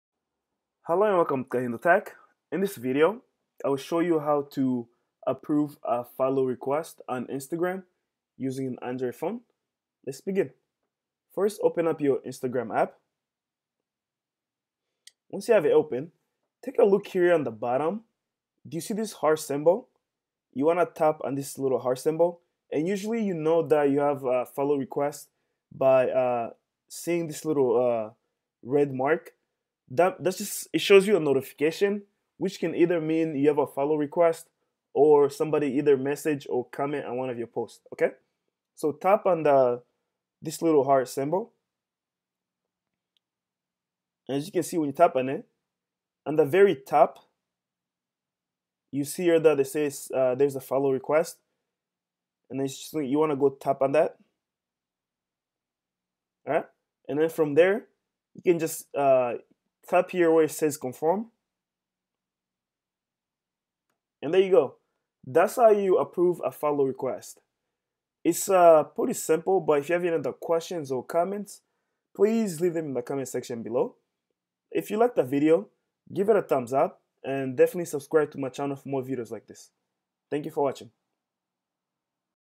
How To Approve A follow Request Instagram Android
This videos How To Approve A follow Request Instagram Android.